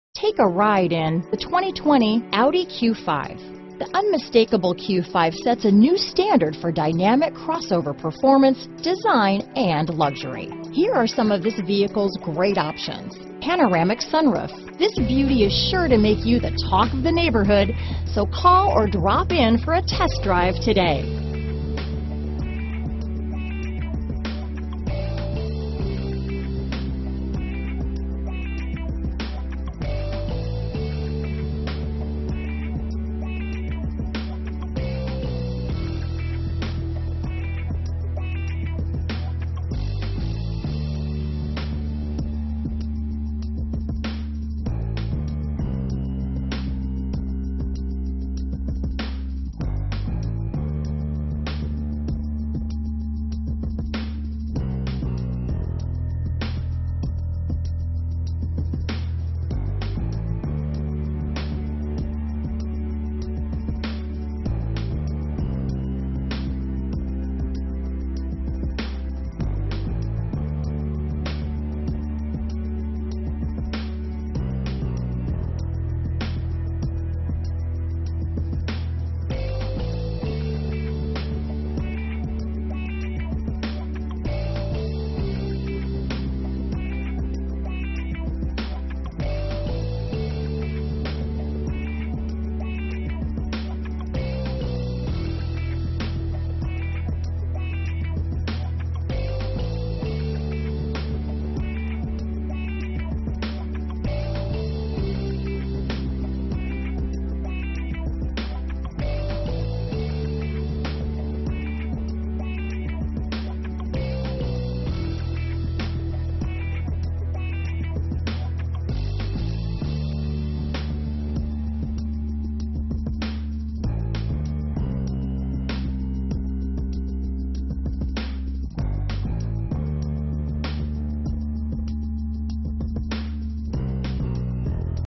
2020 Audi Q5 Summit, Short Hills, Livingston, Westfield, Maplewood, NJ MD200319
Mythos Black Metallic New 2020 Audi Q5 available in Summit, New Jersey at DCH Millburn Audi. Servicing the Short Hills, Livingston, Westfield, Maplewood, NJ area.

PANORAMIC SUNROOF, SUNROOF, 28 MPG Highway, .
PANORAMIC SUNROOF,CONVENIENCE PLUS PACKAGE  -inc: Audi Phone Box  Parking System Plus (Front/Rear Acoustic Sensors),CONVENIENCE PACKAGE CREDIT,CONVENIENCE PLUS PACKAGE CREDIT